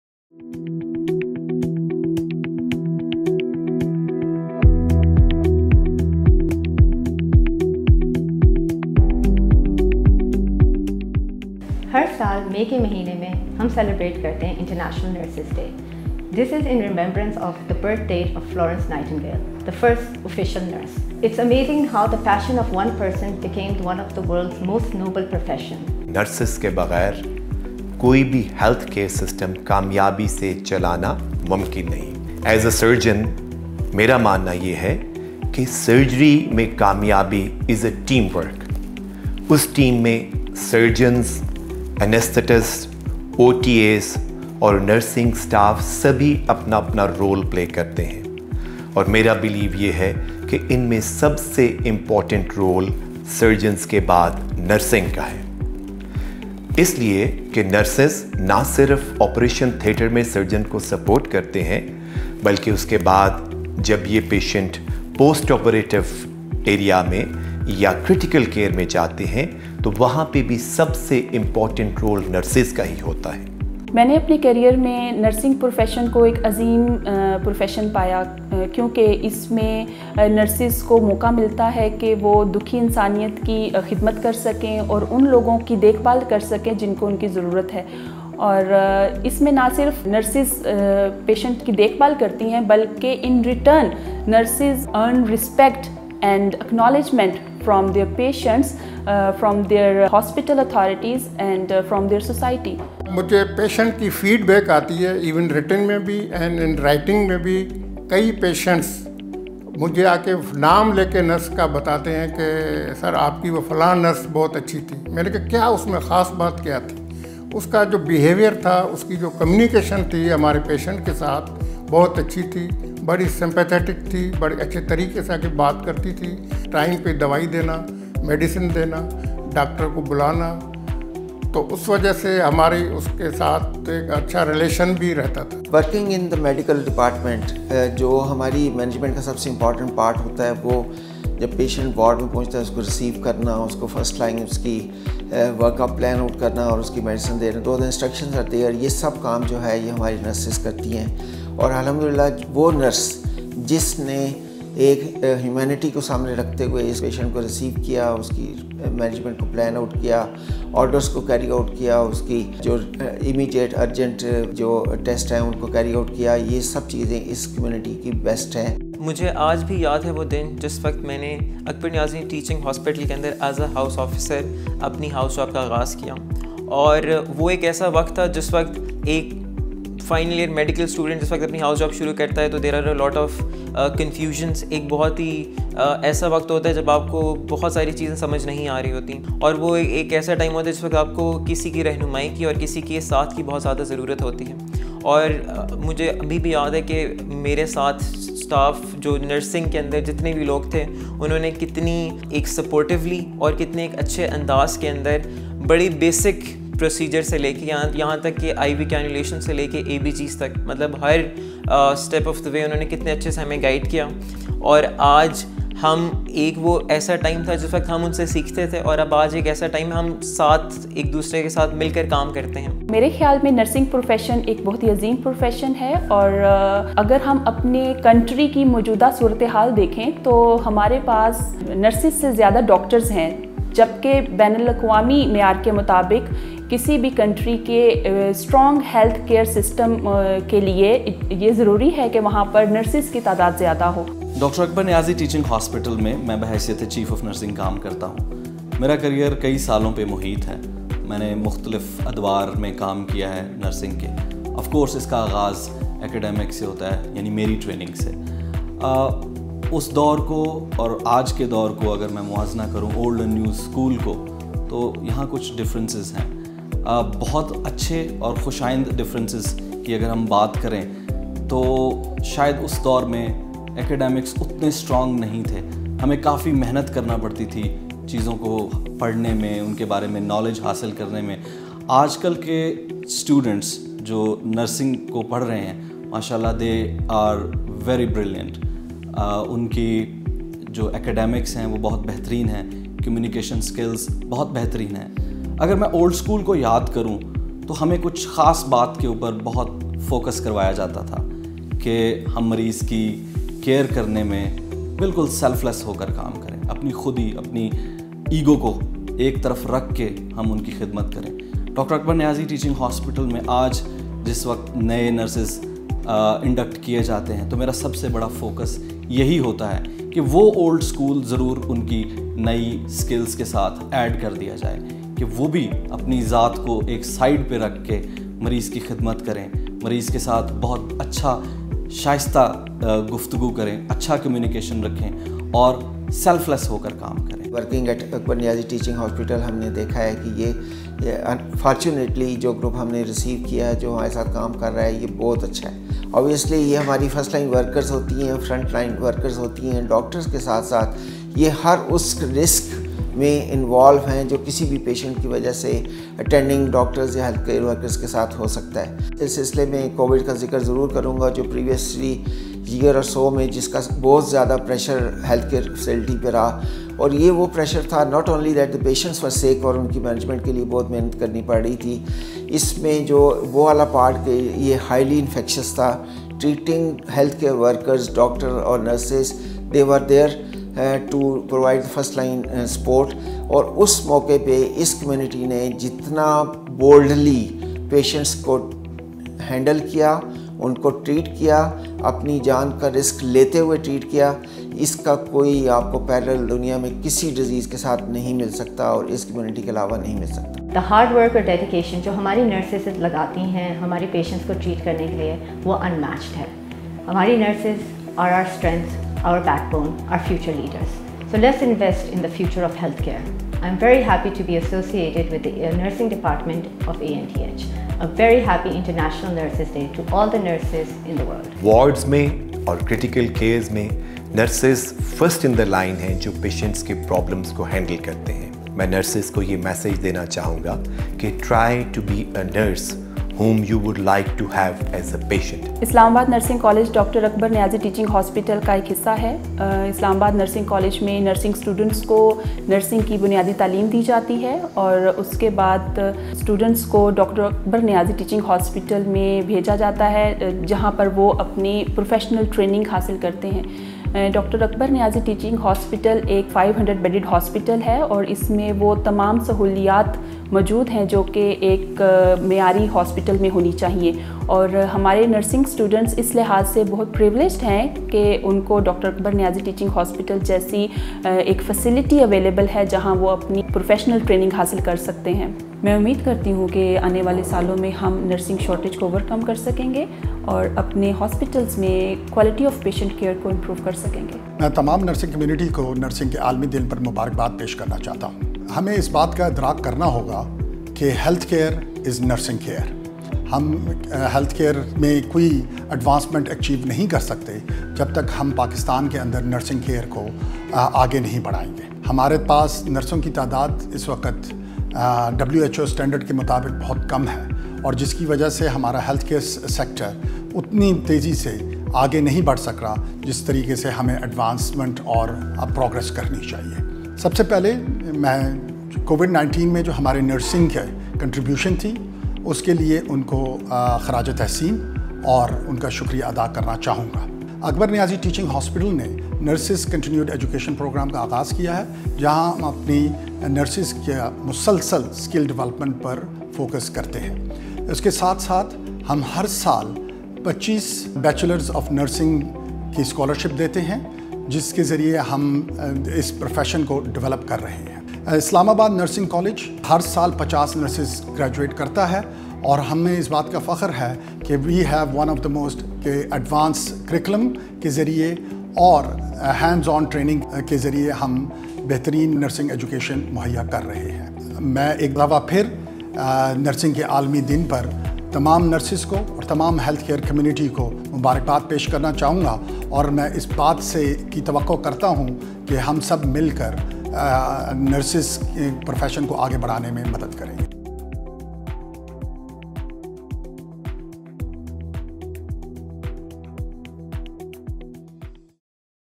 Tribute to Nurses | ANTH & IMDC
One of the most noble professions, nursing is the backbone & strength of any healthcare system. Nurses are the only healthcare professionals that always remain on frontlines having a far-reaching role in dealing with the patients clinically & emotionally.

Nurses are such a lamp that spreads the rays of hope. Without nurses, it’s impossible for a healthcare setup to even imagine giving compassionate care to the patients. Nursing is a profession where you transform your passion into an obligation with dedication & commitment.